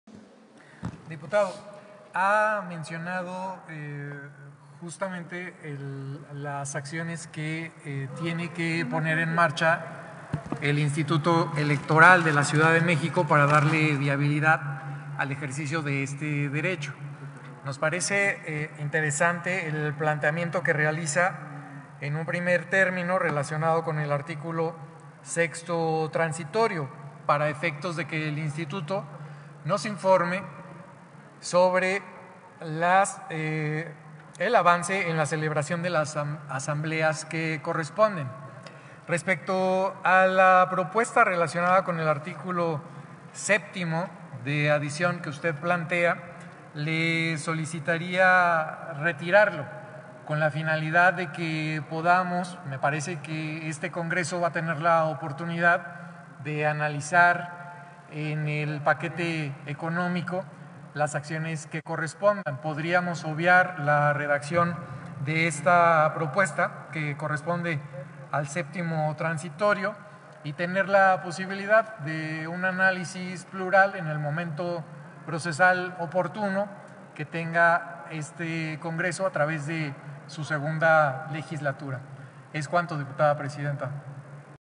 Sesión Extraordinaria 12-08-21
Dictamen que aprueba con modificaciones la iniciativa con proyecto de decreto que adiciona los Artículos Transitorios vigésimo primero y vigésimo segundo de la Ley de Participación Ciudadana de la CDMX, así como los Artículos  décimo octavo y décimo noveno al decreto por el que se expide el presupuesto de egresos de la Ciudad de México.